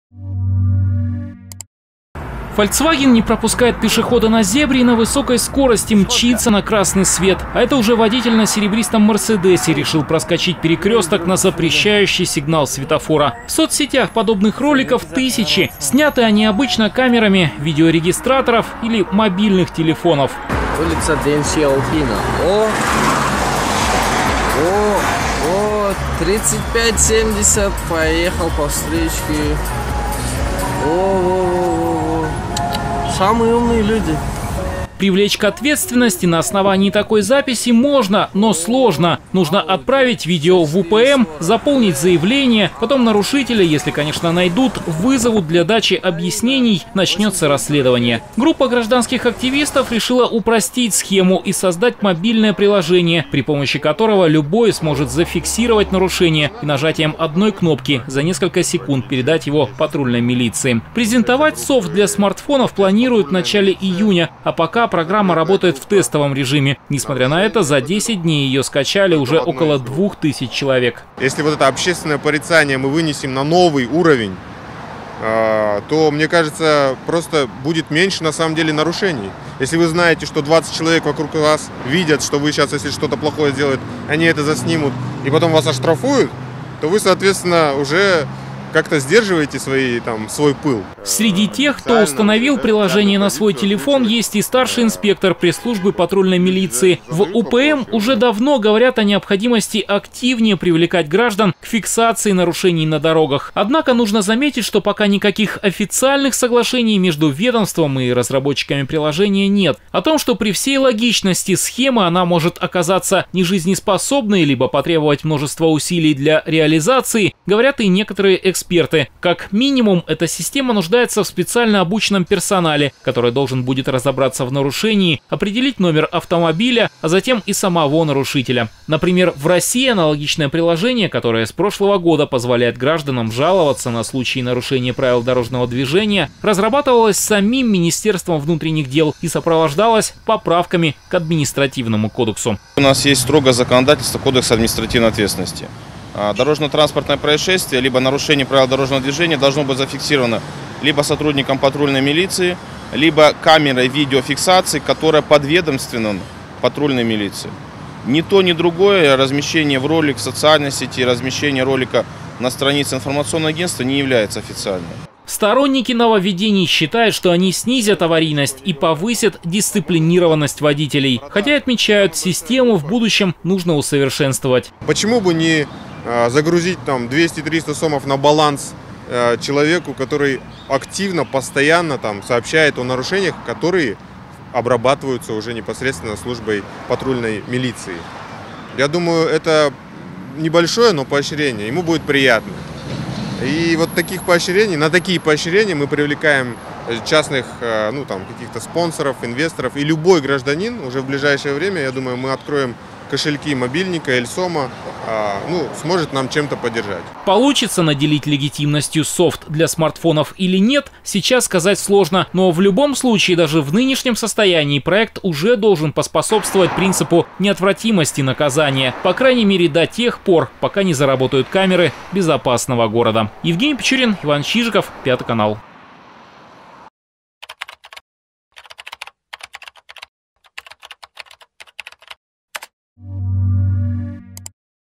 Приложение для охоты на нарушителей - D2KG
Теперь весь авто мир Кыргызстана в одном приложении. Ура!
Приложение для водителей, где можно найти все. Вливайся и добавляй своё авто.

Забудь про кучу групп в WhatsApp. Вот ссылка на приложение D2KG в Play Market